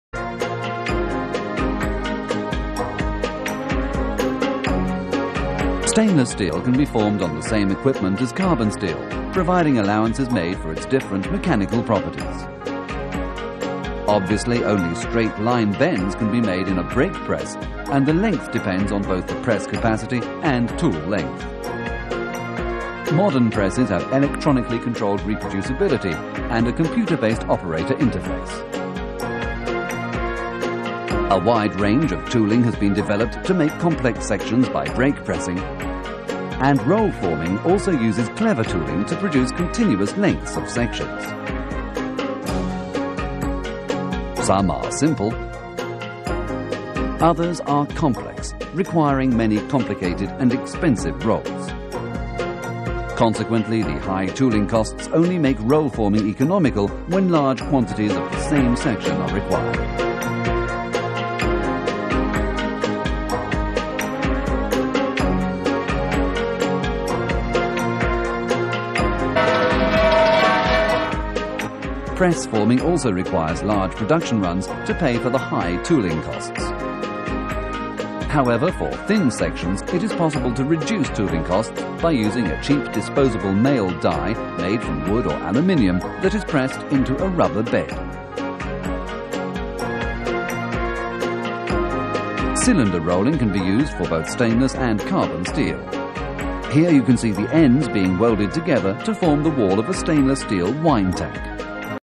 Stainless Steel Against Corrosion_PART 6-3-FORMING
Stainless Steel Against Corrosion:
- The cost of corrosion
- Oxidation and corrosion in metals
- Corrosion resistance of stainless steel
- Types of stainless steel
- Forming and joining